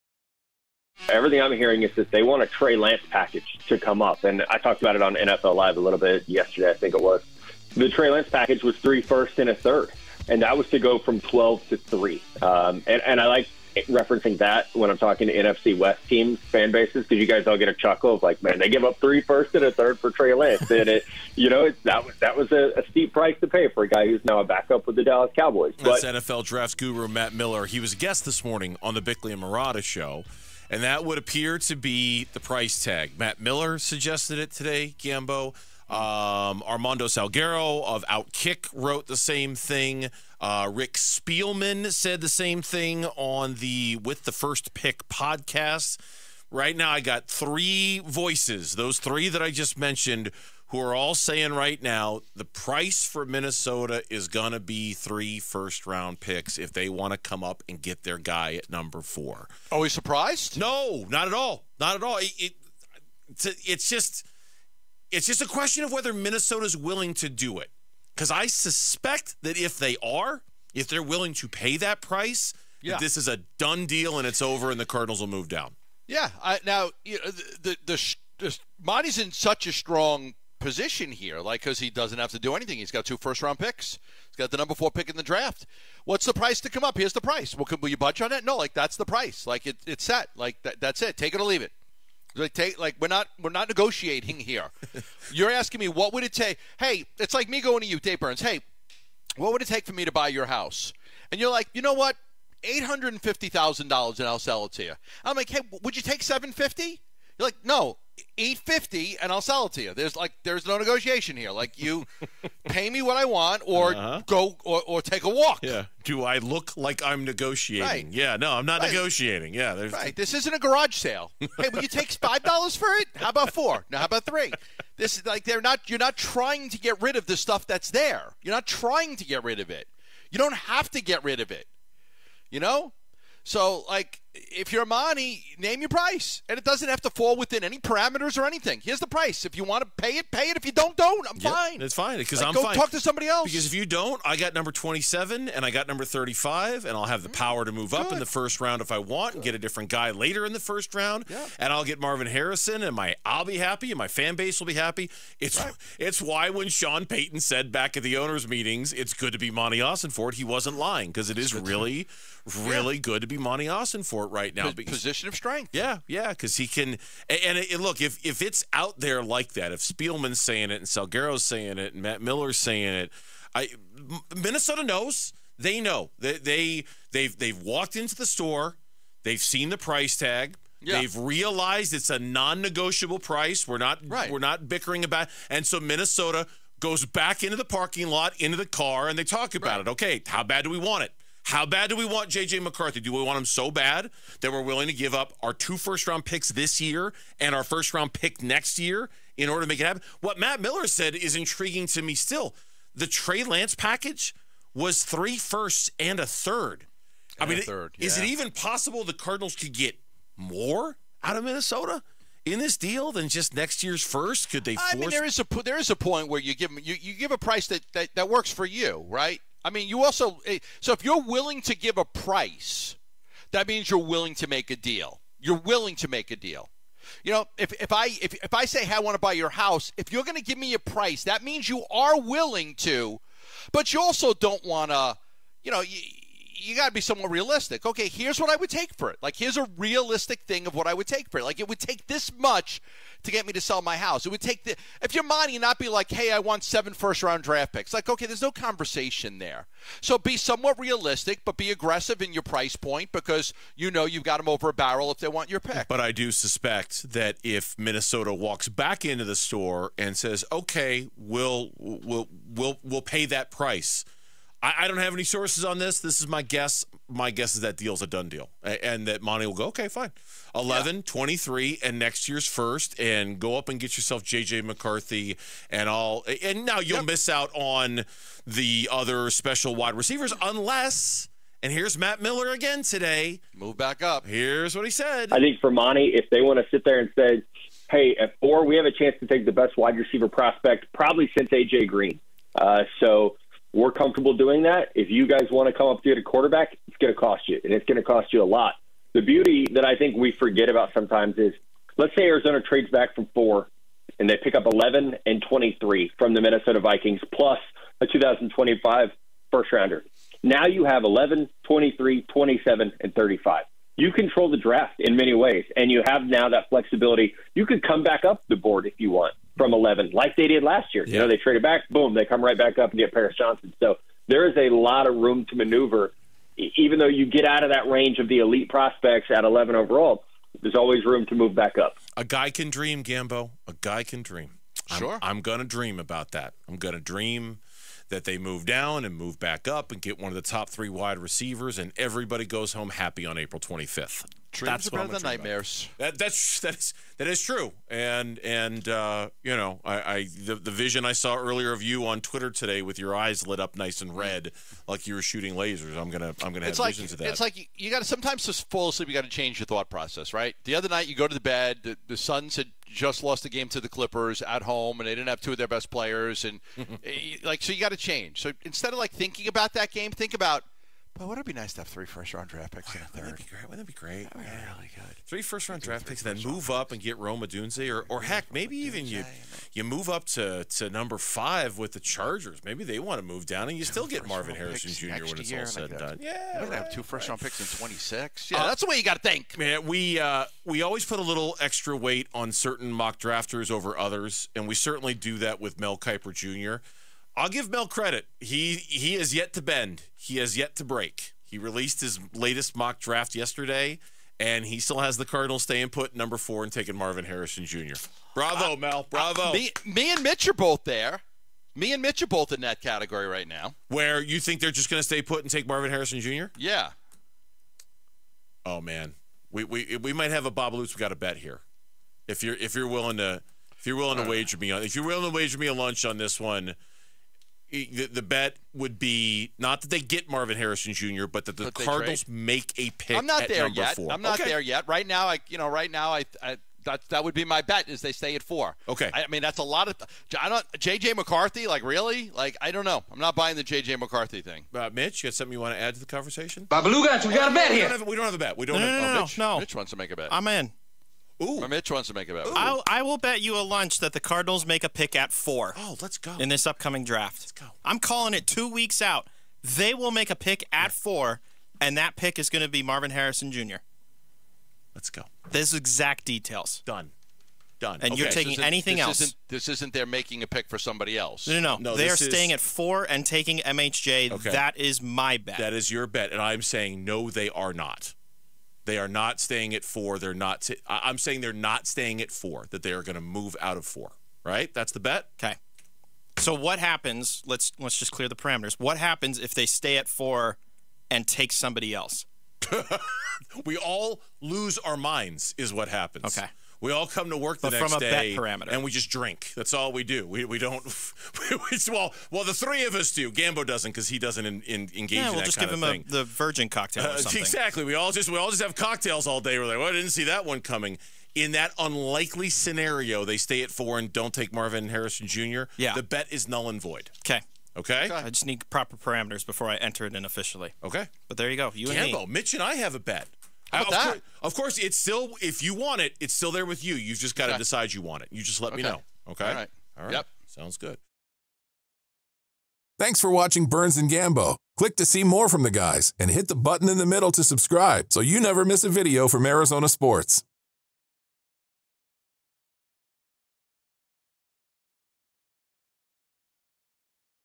Arizona Cardinals in good spot with No. 4 pick in 2024 NFL Draft
The Arizona Cardinals have a lot of leverage when it comes to their No. 4 pick in the 2024 NFL Draft. Burns & Gambo talk about what could be if other teams give GM Monti Ossenfort a call about it.

Video: Felisa Cárdenas and Ed Cole/Arizona Sports
Photo Credit: John Fisher/Getty Images